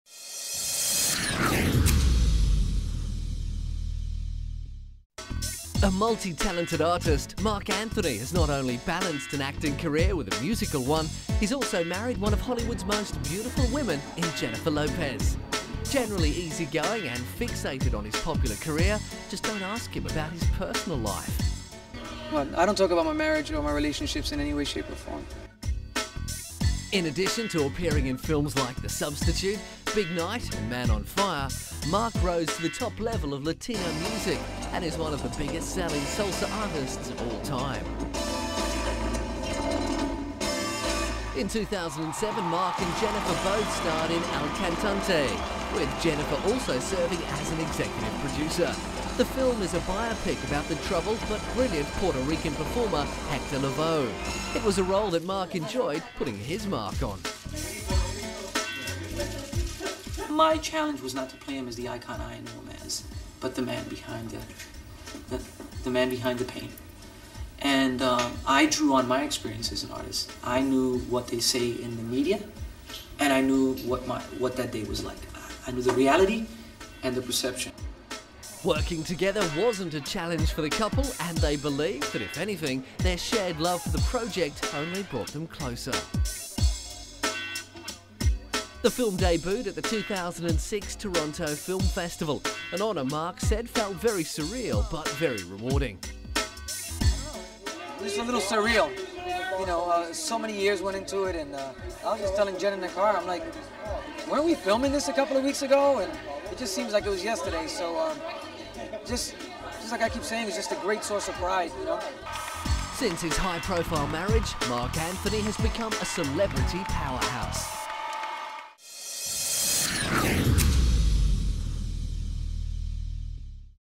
Mark Anthony & Jennifer Lopez Are The Best Hollywood Couple- HQ
Hey Guys!! Check out singer Mark Anthony speak about his marriage to the sexy Jennifer Lopez on the red carpet!!